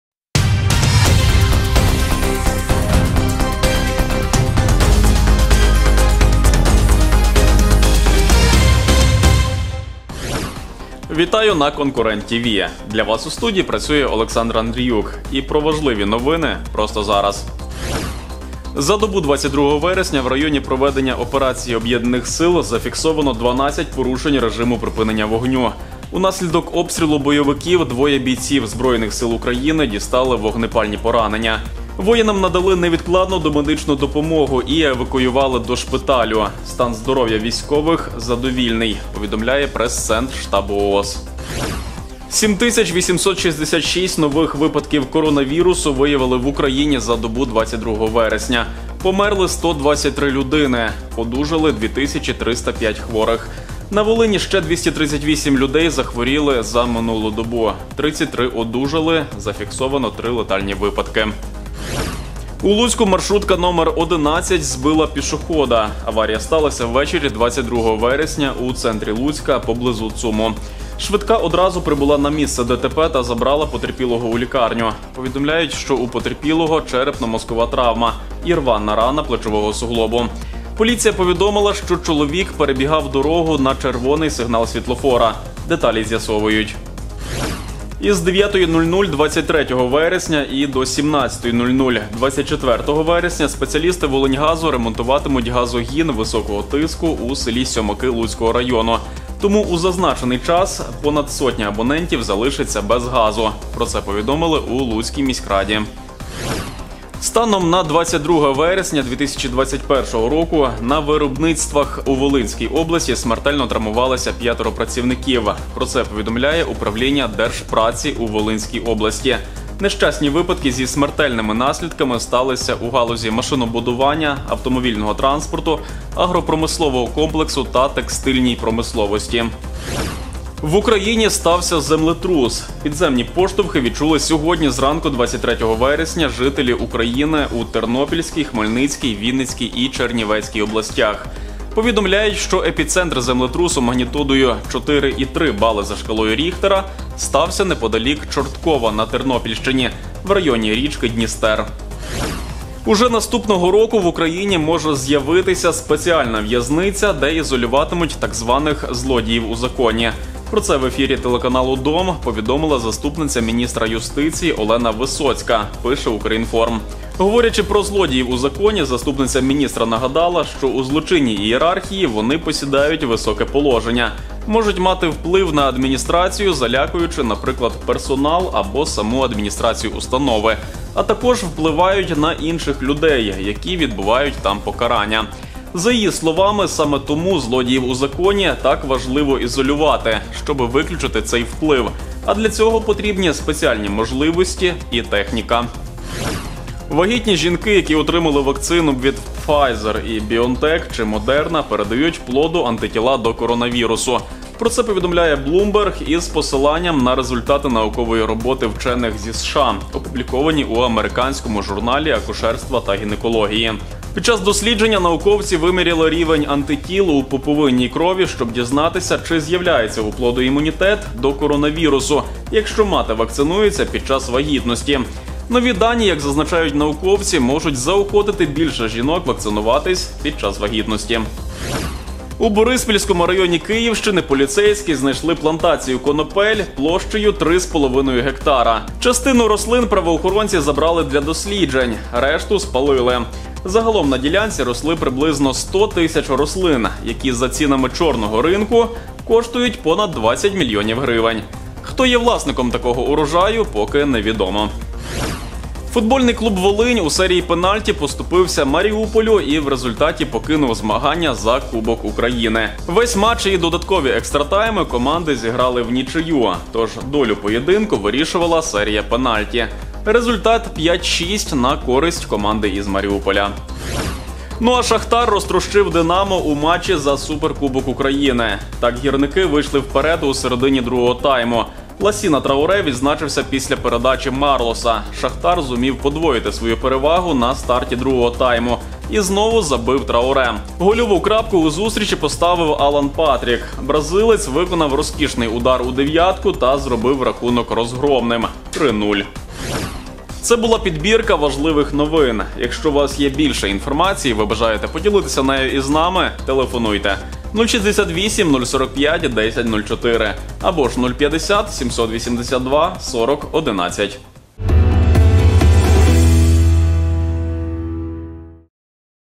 Новини на Конкурент TV: маршрутка збила чоловіка, стався землетрус, «Волинь» поступилася «Маріуполю»
Коротко про важливе! 23 вересня! 11.30!